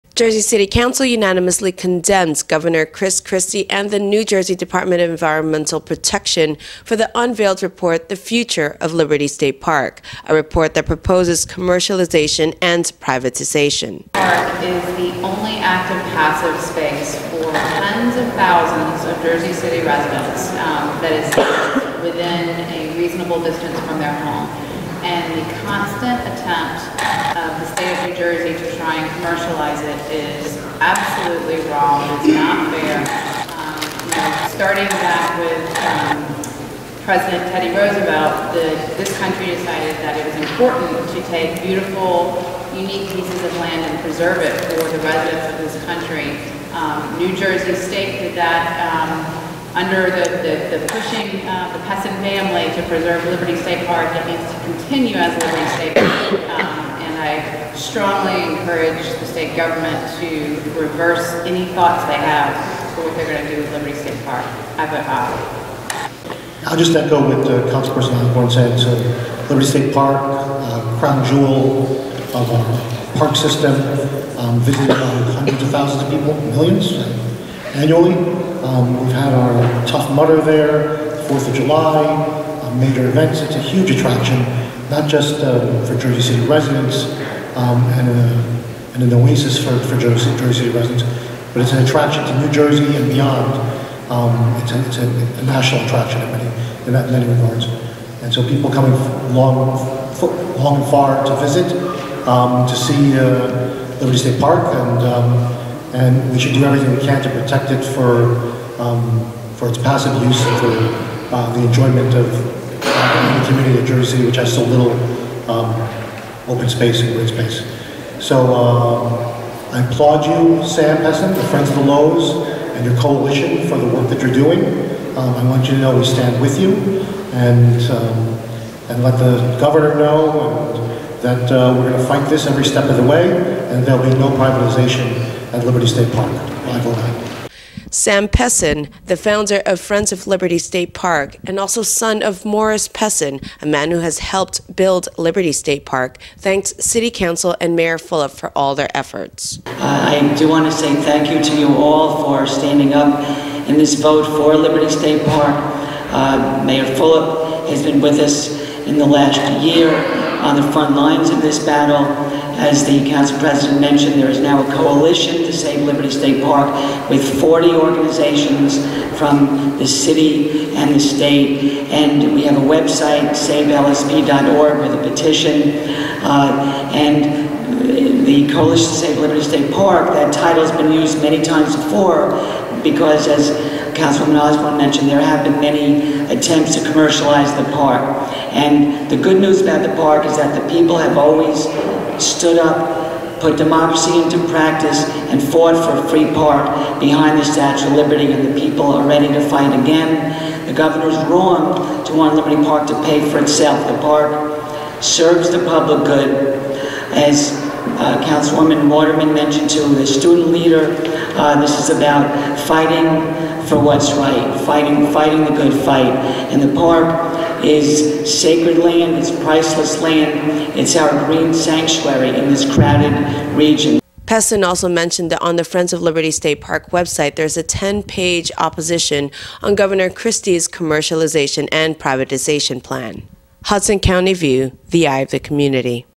Jersey City council opposes LSP privatization
Joining Jersey City Mayor Steven Fulop and state Senate President Steve Sweeney (D-3), the Jersey City council condemned the Gov. Chris Christie administration for trying to privatize Liberty State Park.